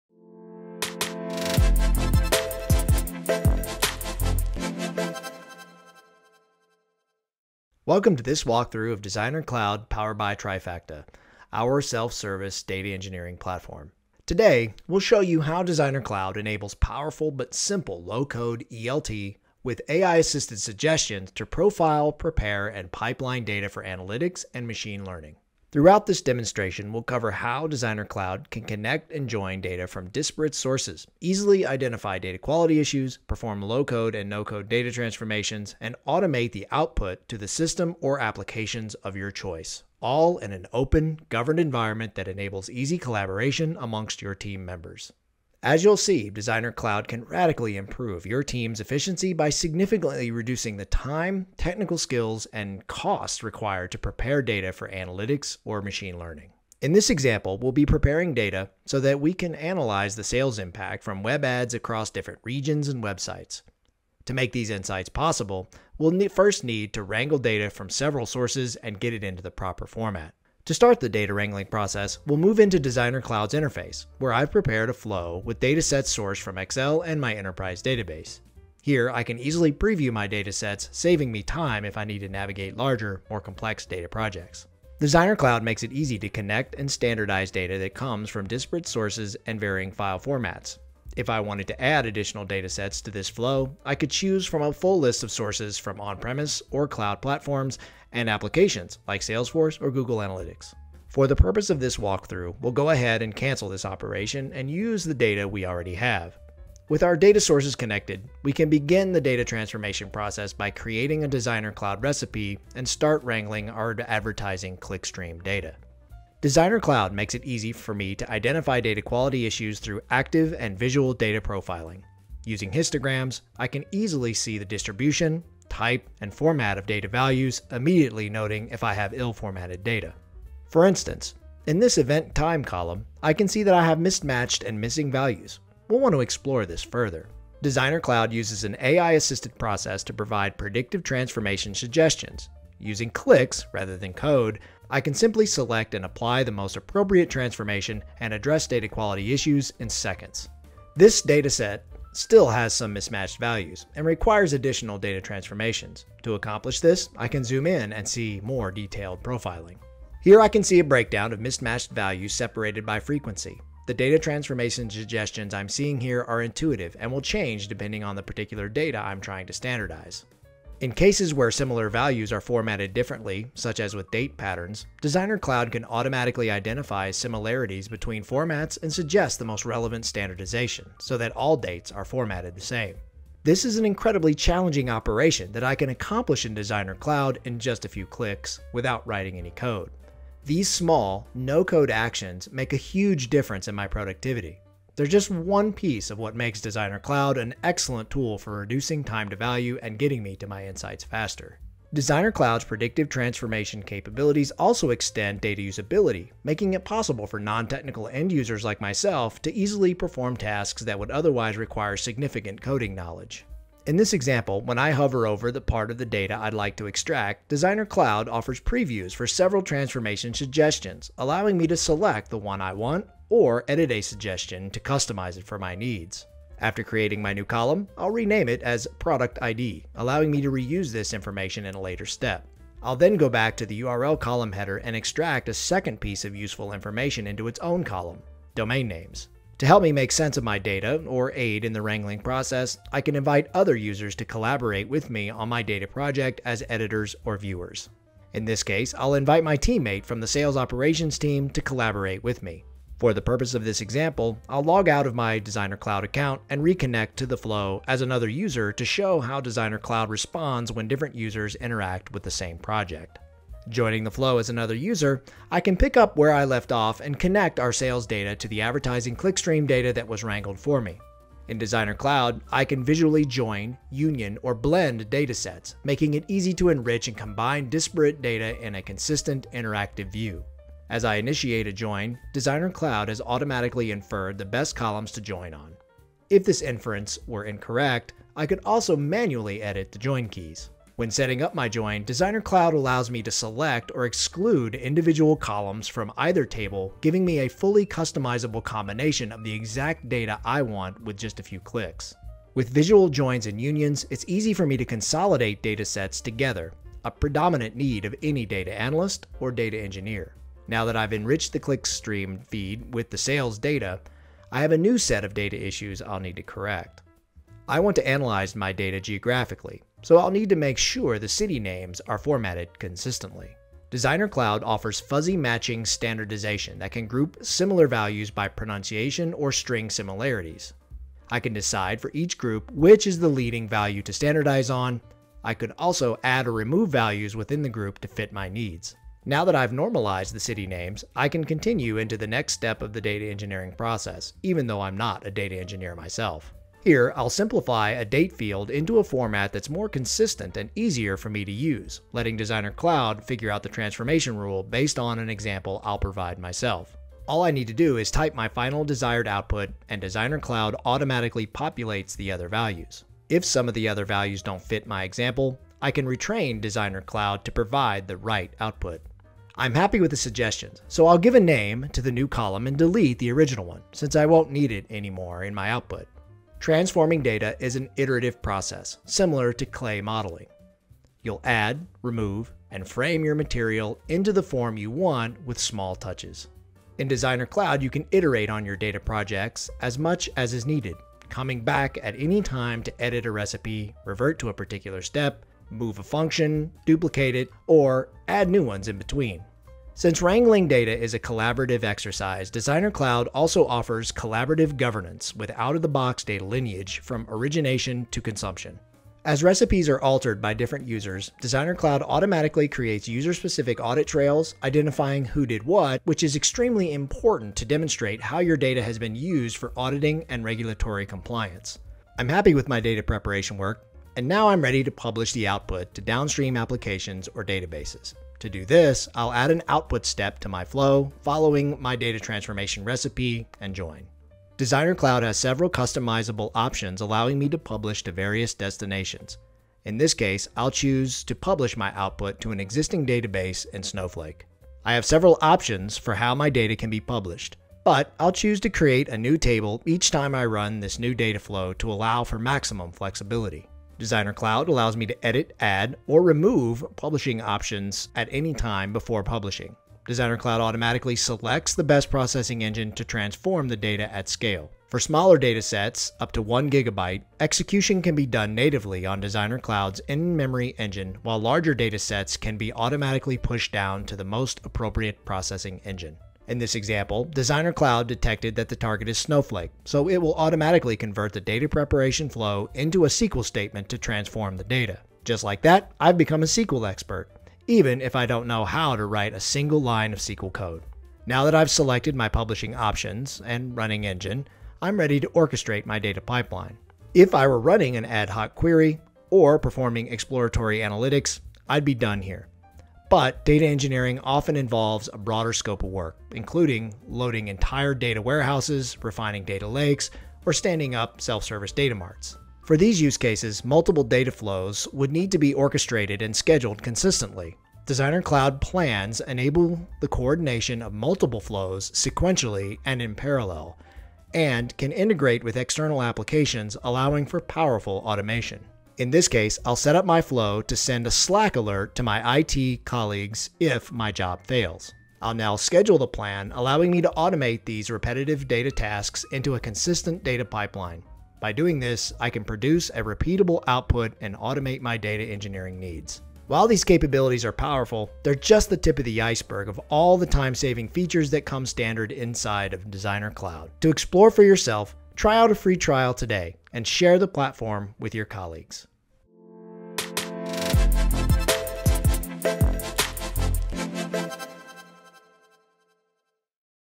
Designer Cloud powered by Trifacta - Demo - Accelerate Data Engineering for the Cloud
Transform your data, ensure quality, and automate data pipelines at any scale. See our product in action in this walkthrough video demonstrating how you can profile, prepare and pipeline data for analytics and machine learning.